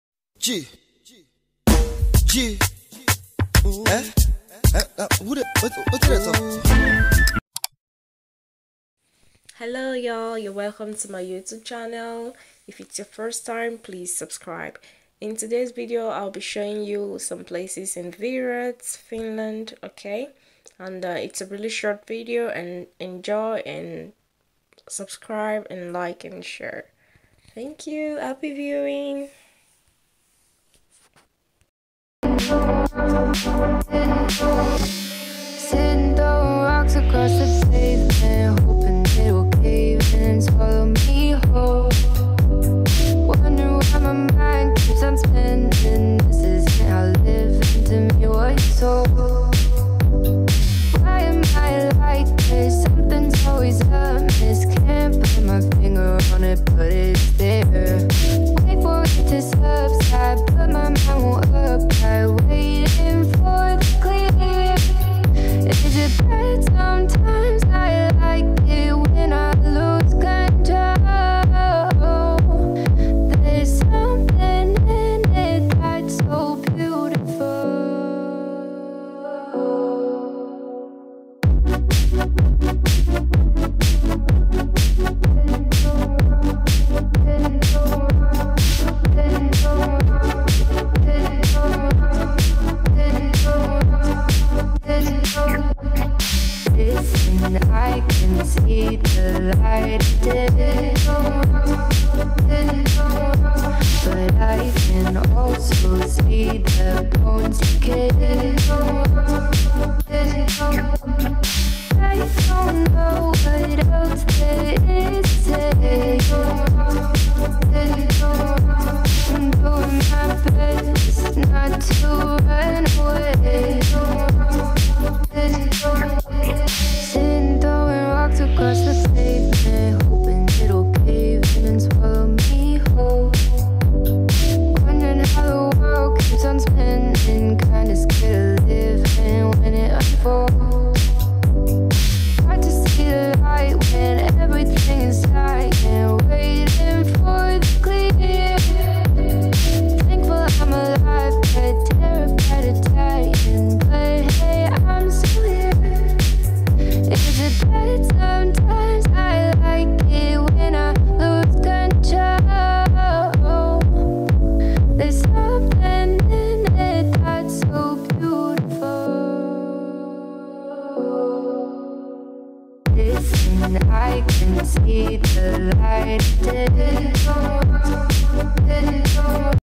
DRIVE WITH ME AROUND VIRRAT FINLAND | FINLAND YOUTUBER
Video of a short drive around Virrat Finland, Enjoy!